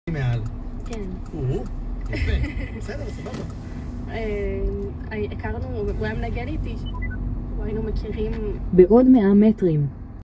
MAN NL323F of Egged on Route 17 on Rotschild st. in Rishon LeZion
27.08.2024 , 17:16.

מאן NL323F של אגד מבצע קו 17 (ממכון וולקני ראשון לציון לחניון מוזיאון אגד חולון) ברח' רוטשילד/דב הוז ראשון לציון (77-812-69, #04028, מסניפי גוש דן (בעבר השתייך לסניפי חיפה וסניף הנגב).